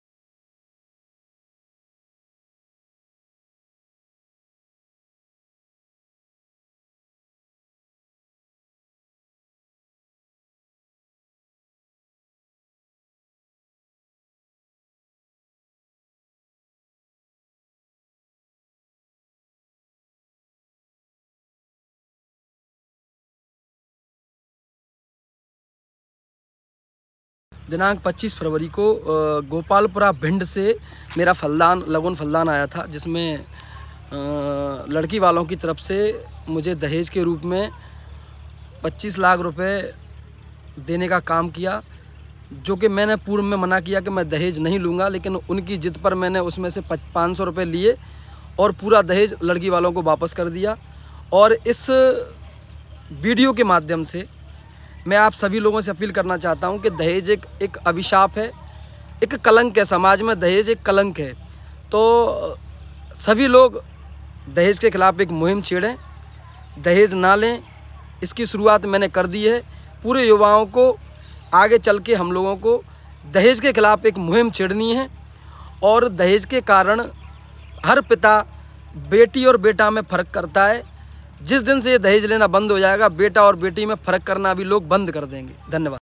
दूल्हे ने ठुकराया 25 लाख दहेज। केवल 500 ₹ का नोट माथे से लगाकर शेष राशि वापस की।
सबलगढ़ - सबलगढ़ तहसील के ग्राम अटार में दिनाक़ 25 फरवरी 2024 को भिण्ड जिले से सिकरवार परिवार में फलदान टीका आया था,
 अटार गांव के सरपंच ऊधम सिंह सिकरवार के लड़के एवम भाजपा के वरिष्ठ नेता समाज सेवी सौरभ सिंह सिकरवार का रिश्ता भिण्ड से तय हुआ था ।जब सौरभ का फलदान और टीका चढ़ाया जा रहा था। तब बेटी बालो की तरफ से 25 लाख रुपए नगद राशि दूल्हे के हाथो में रखी , तभी दुल्हा बने सौरभ ने थाल में रखे पच्चीस लाख रुपए में से एक पांच सो रुपए का नोट उठाकर अपने माथे से लगा लिया और पूरे रुपए बेटी बालो को वापस कर दिए जब इस संबंध में बेटी बालो ने कहा तो सौरभ ने कहा कि में दहेज नहीं लूंगा इस फलदान टीका कार्यक्रम में भाजपा के वरिष्ठ नेता और विधानसभा अध्यक्ष नरेंद्र सिंह तोमर स्वयं उपस्थित रहे, उनके साथ जिला पंचायत अध्यक्ष आरती गुर्जर भाजपा जिलाध्यक्ष योगेश पाल गुप्ता पूर्व विधायक रघुराज सिंह कंषाना सत्यपाल सिंह सिकरवार भी उपस्थिति रहे इस बात की चर्चा पूरे क्षेत्र में फैल गई जब इस संबंध में दूल्हा सौरभ से बात की तो उन्होंने कहा कि मैं दहेज विरोधी हूं जब मैं अन्य लोगों से कहता हूं कि दहेज प्रथा एक अभिशाप और बुराई है तो में कैसे दहेज ले सकता हूँ।